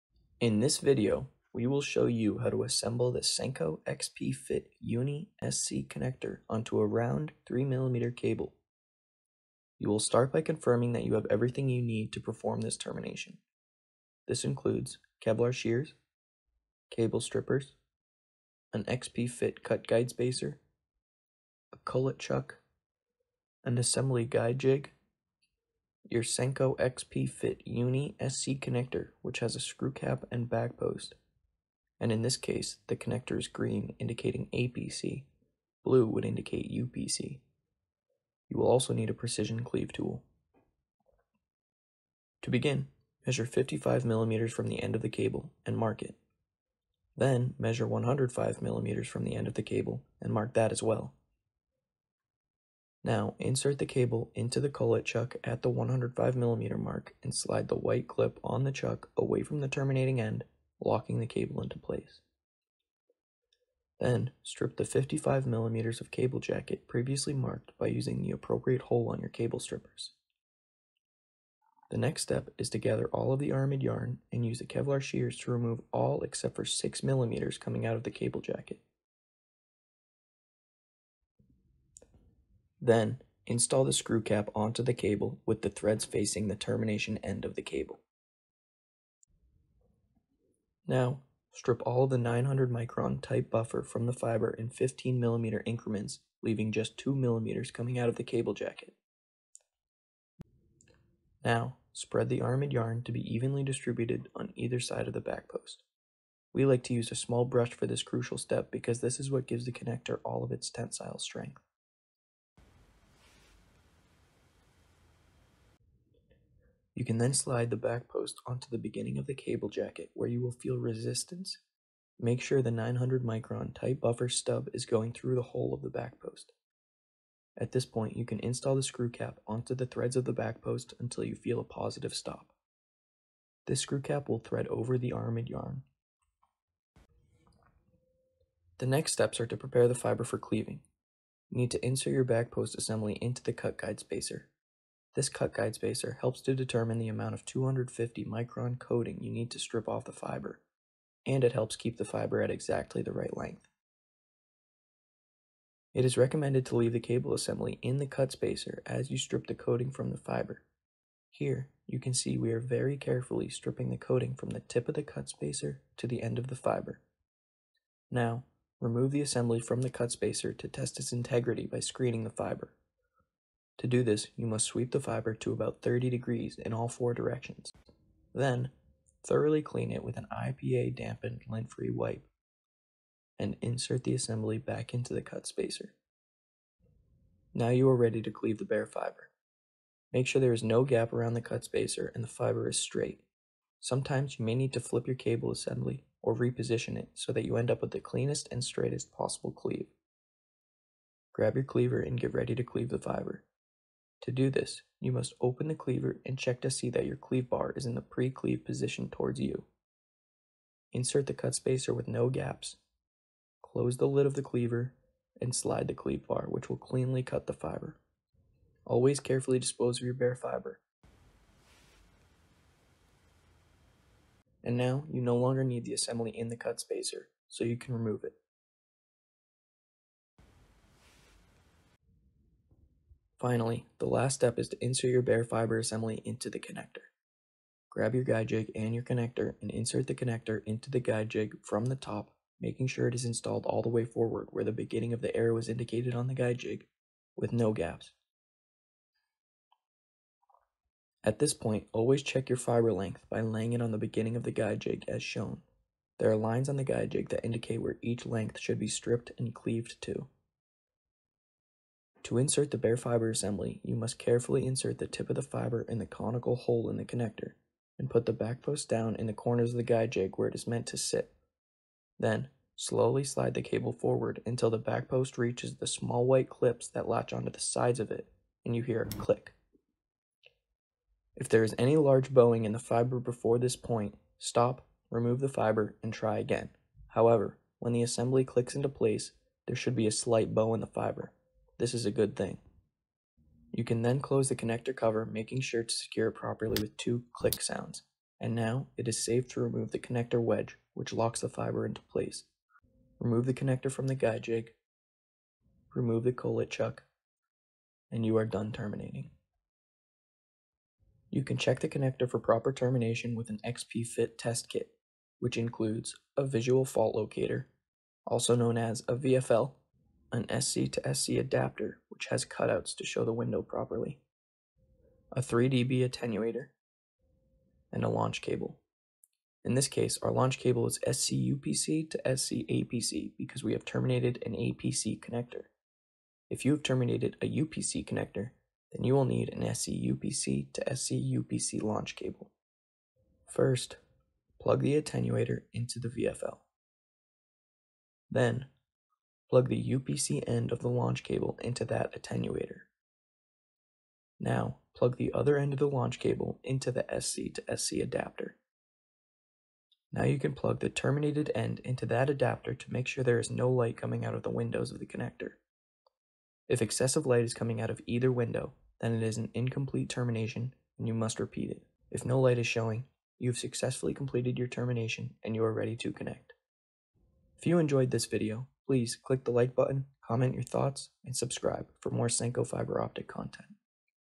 How to Terminate XP Fit Uni SC on to 3mm Cable
For your safety, it is always advised to follow proper fiber optic handling techniques and utilize the correct protective gear when performing terminations. Questions about safety?